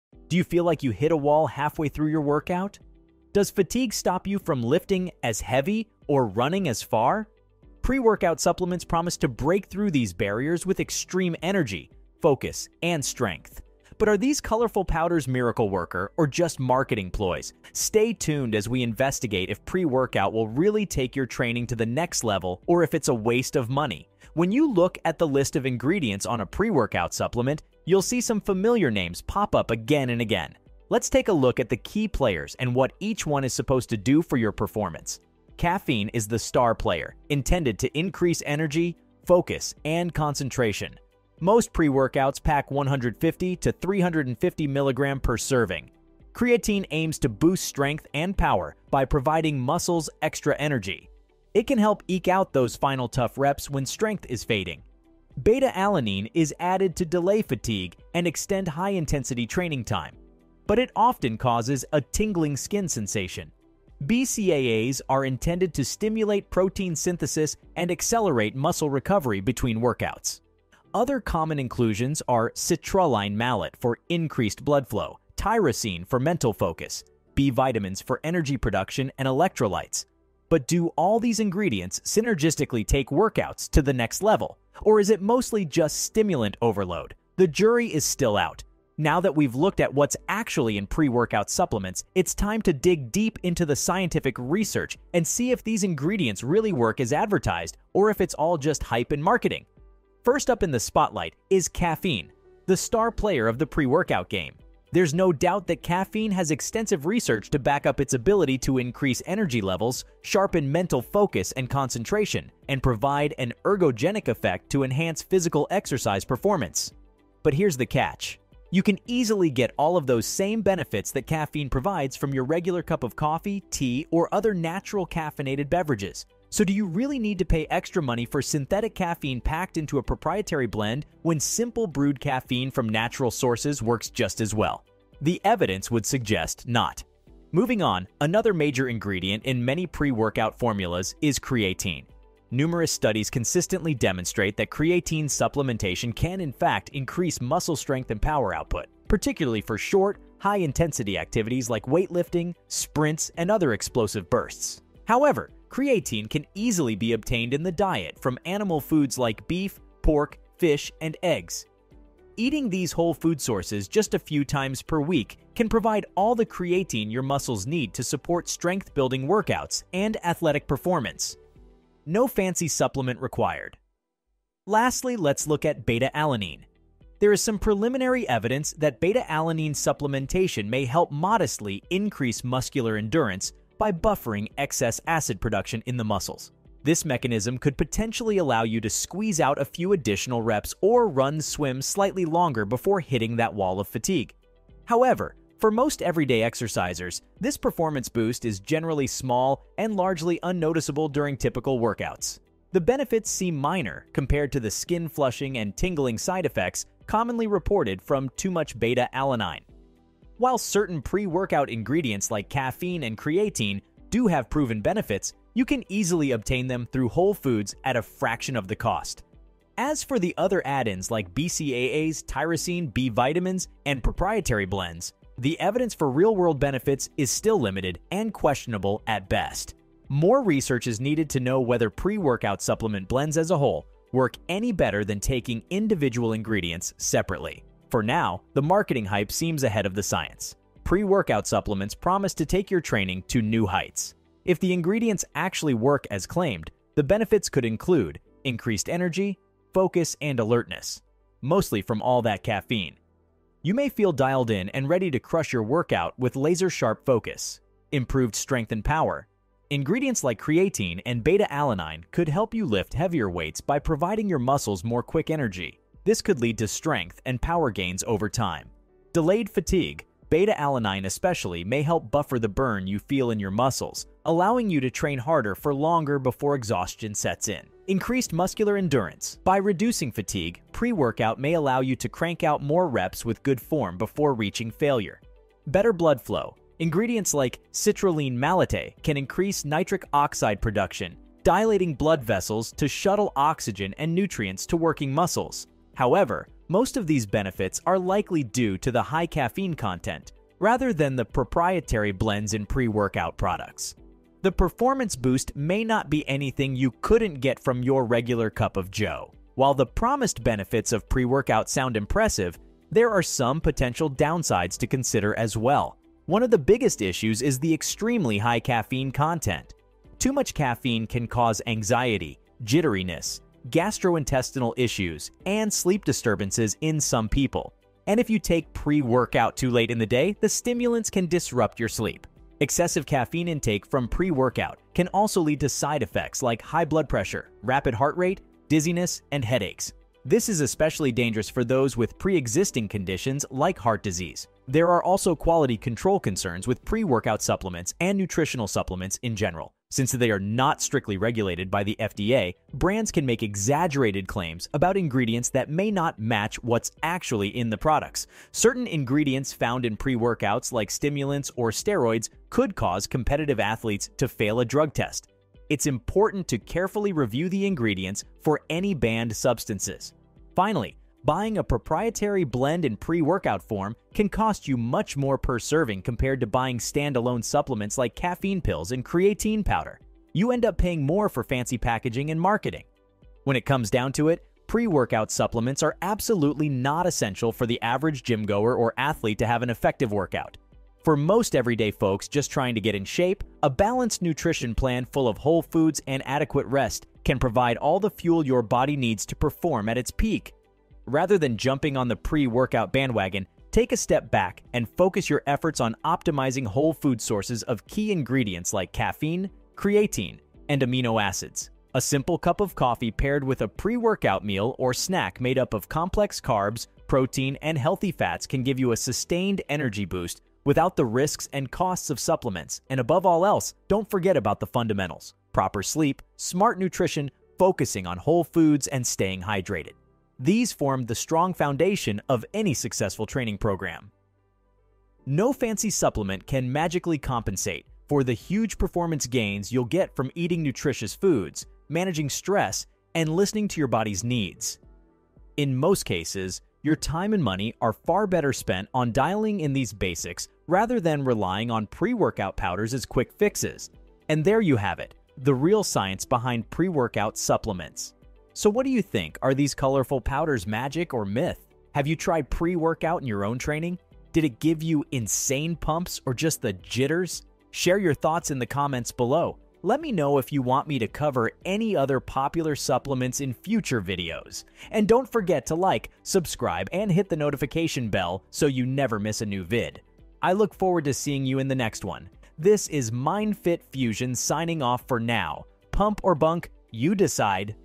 Do Pre-Workout Supplements Really Work or Are They a Waste?
Ever wonder if those flashy pre-workout supplements are the real deal or just smoke and mirrors? Join us as we dive deep into the world of pre-workout powders to uncover the truth.

In this video, we scrutinize the ingredients that make up pre-workout supplements and examine their purported benefits. From caffeine's energy boost to creatine's strength enhancement, we break it all down. But here's the twist – could you be getting these benefits from your morning coffee and regular diet instead?

We also explore the potential downsides of pre-workout supplements, from excessive caffeine intake to quality control concerns and cost. Is it really worth the hype and the price tag?

Discover the real science behind pre-workout supplements and make an informed decision about whether they're right for you. Share your experiences and thoughts in the comments below. Let's separate pump from bunk!